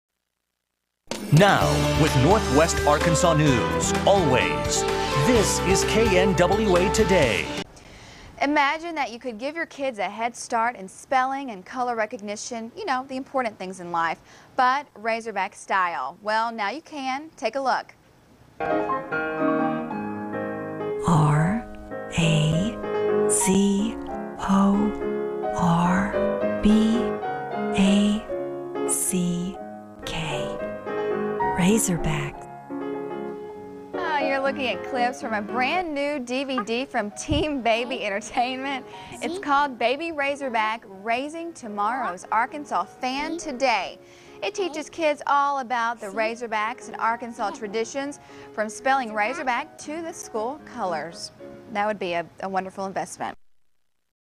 News talks about Razorback Babies
Local news talks about Baby Razorback and how it is a good investment.  From Team Baby Entertainment